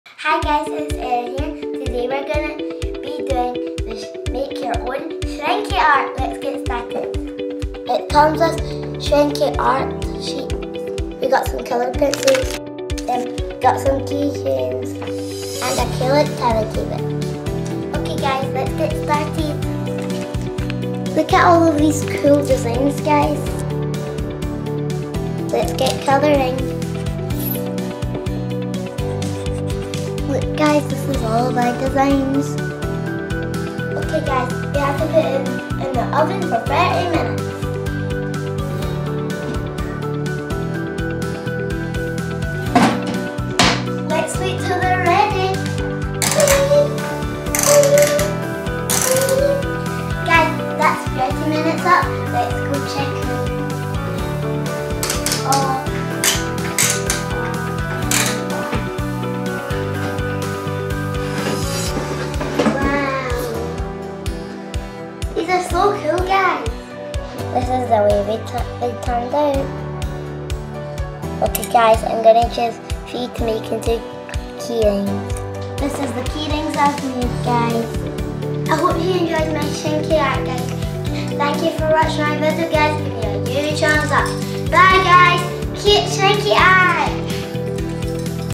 Erin plays with Shrinky Art
Erin plays with Shrinky Art and makes keychains with this fun and easy arts and crafts set for kids